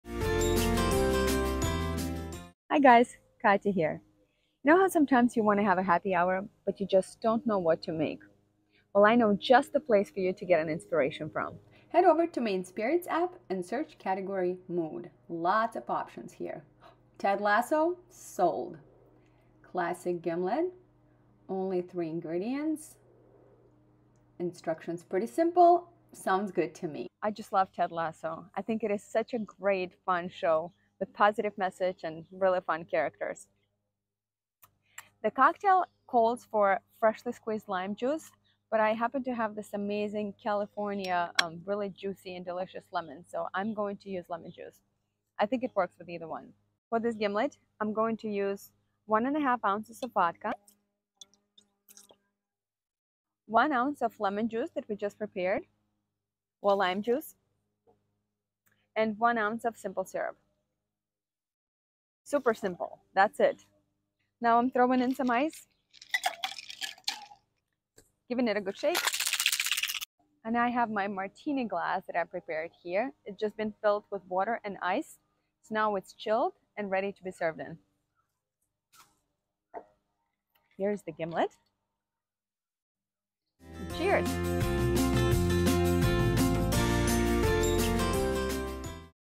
Cocktails with Katya - Classic Gimlet
Join Katya in making a cocktail based on one of her favorite
TV shows 👀⚽️